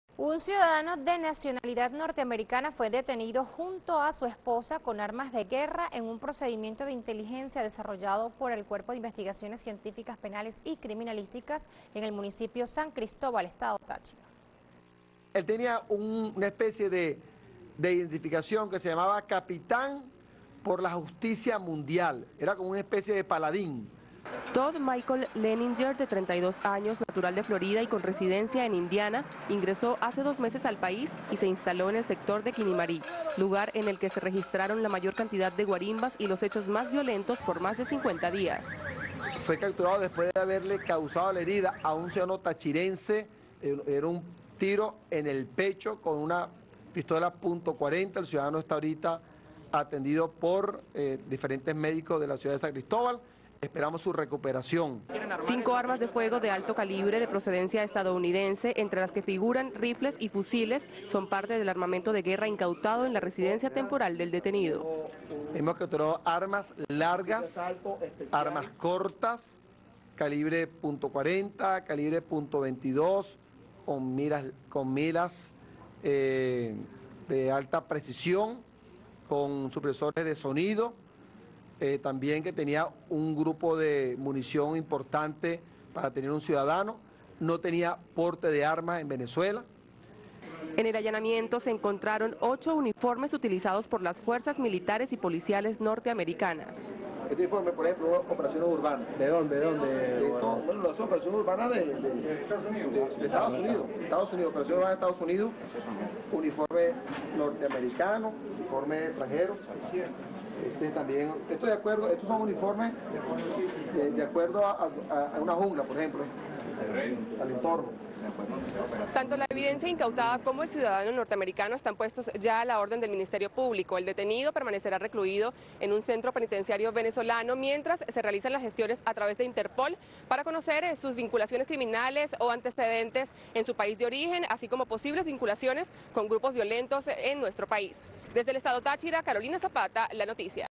Estadounidense detenido en Táchira con armas de guerra, denuncia Vielma Mora
Rueda de prensa del gobiernador de Táchira este 22/04/14